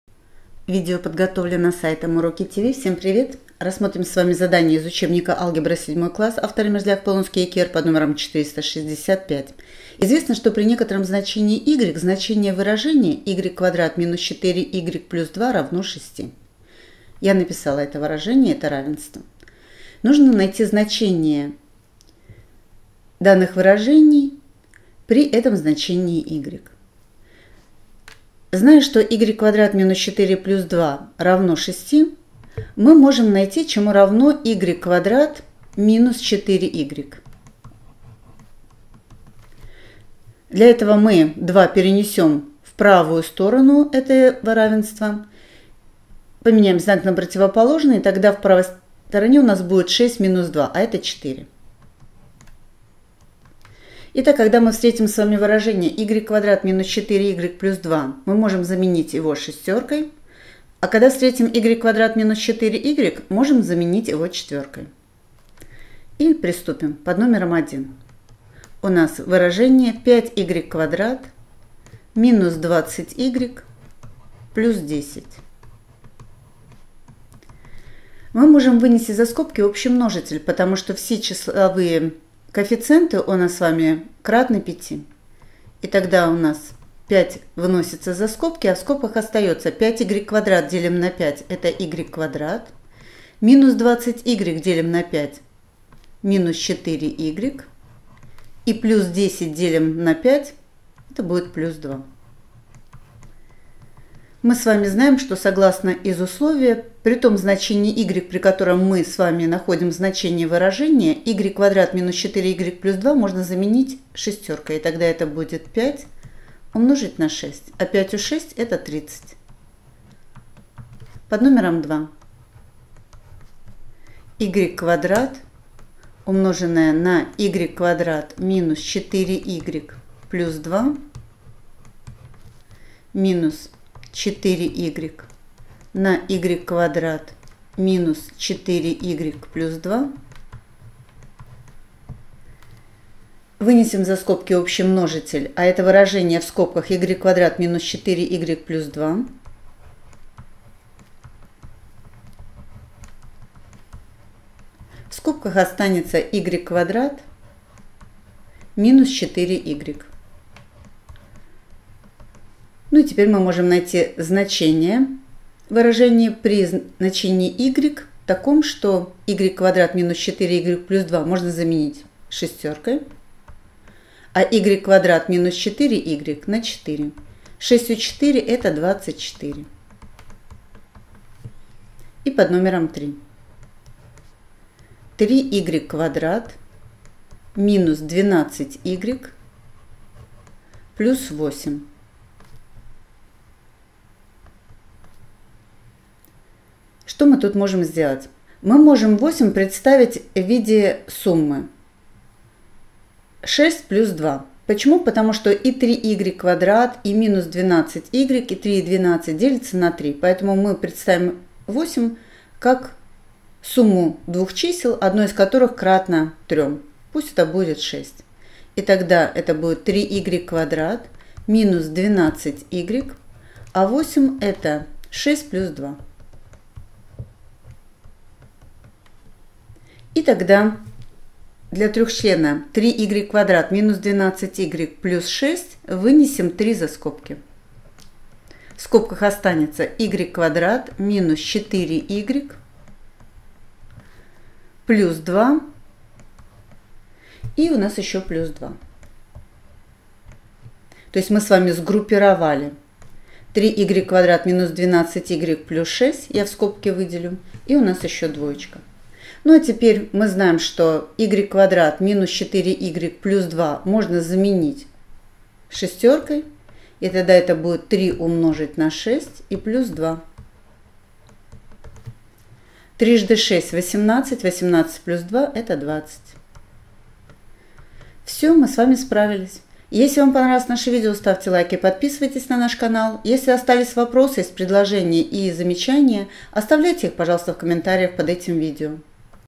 Задание №465 - ГДЗ по алгебре 7 класс (Мерзляк А.Г.)
Чтобы выполнить задание № 465, в котором требуется при данном условии, найти значение выражения, используя вынесение за скобки общего множителя, нужно знать материал 2 параграфа 12 главы, расположенный на страницах 77-79. А еще необходимо вспомнить материал 6 главы на стр. 40-43.